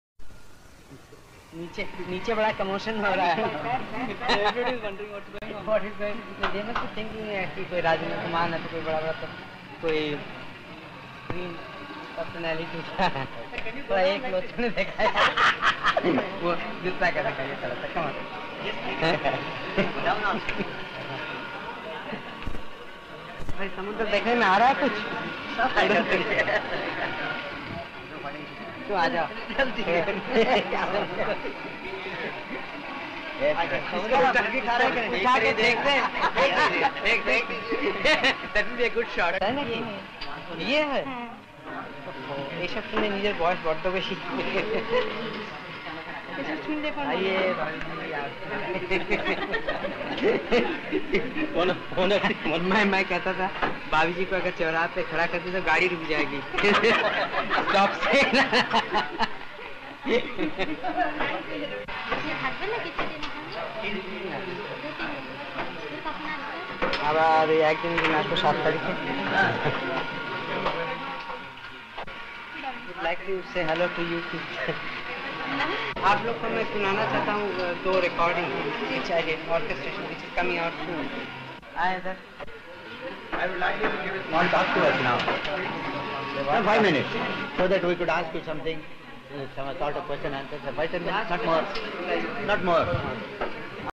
Humor King! - Pandit Ravi Shankar (Jokes) 🤣
The Maestro was known for his humour & jokes! He could make you smile & laugh in any moment! Once someone asked why his name always preceded with the title 'Pandit' then he answered he always stays in a 'pun' mood so that's why people call him as 'Pandit'